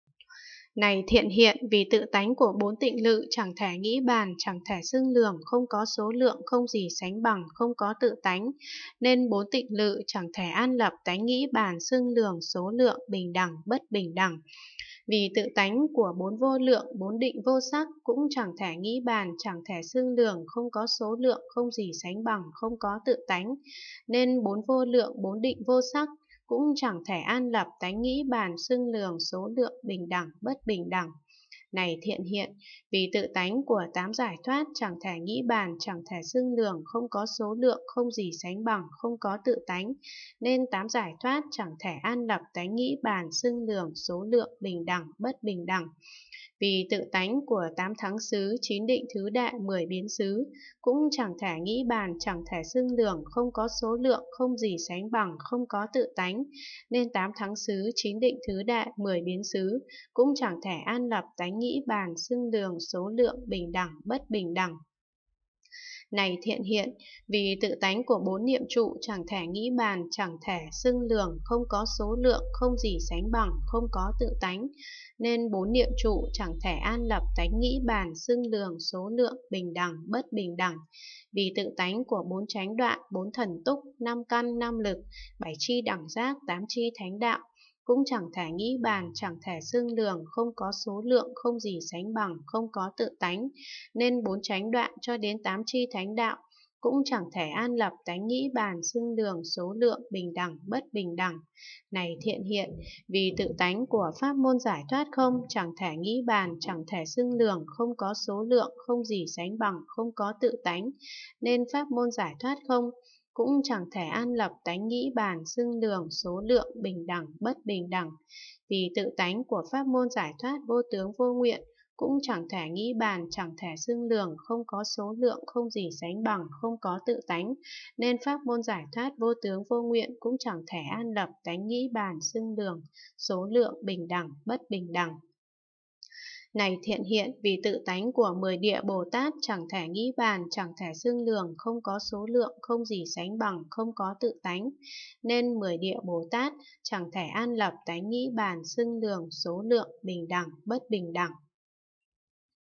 Quyển 309: Kinh đại bát nhã ba la mật đa - Phẩm chẳng nghĩ bàn (rất hay) (309/600) (9)
Phẩm 42: Phẩm chẳng nghĩ bàn
Hán dịch: Pháp sư Huyền Trang
Việt dịch: Hoà thượng Trí Nghiêm
Giảo chính: Hoà thượng Quảng Độ